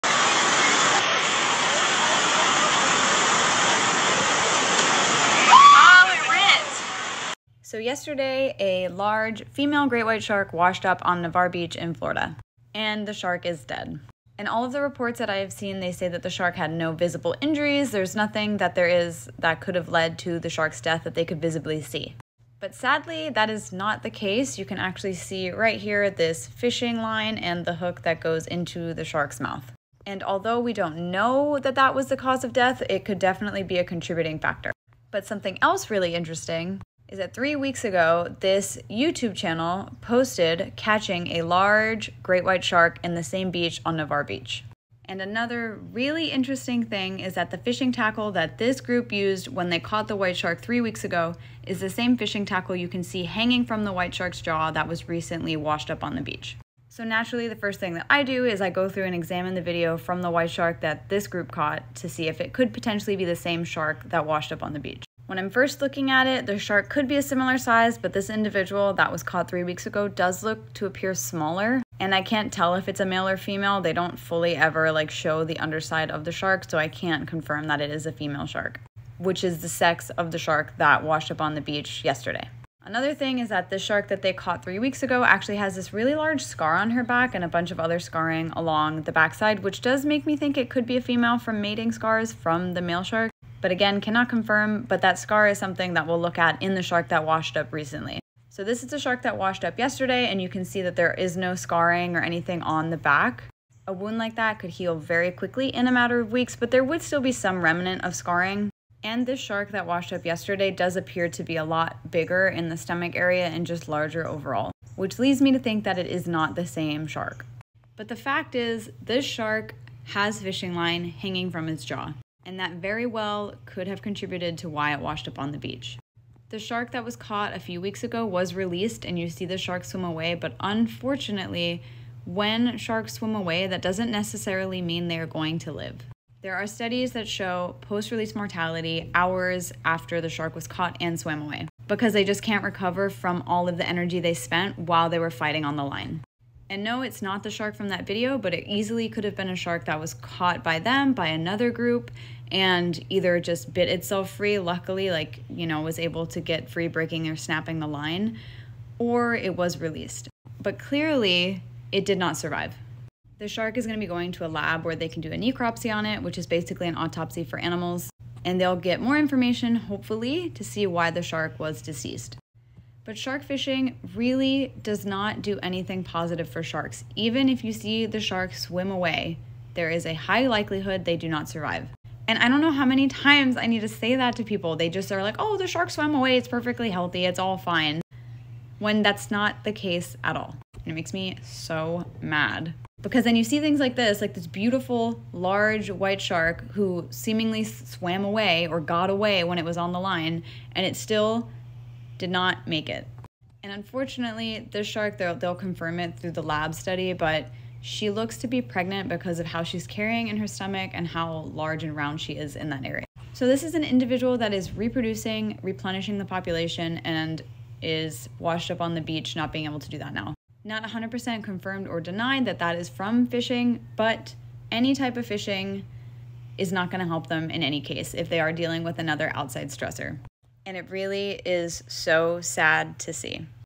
Marine Biologist Reacts To Great White Shark Washing Up On Florida Beach!
This white shark was found on Navarre Beach in Florida yesterday washed up no longer alive. News reports state that the shark had no obvious signs of what could have caused its demise, but there is apparent fishing tackle hanging from its jaw. A shark fisherman also caught a great white shark of a similar size three weeks prior to this shark washing up. What do you guys think?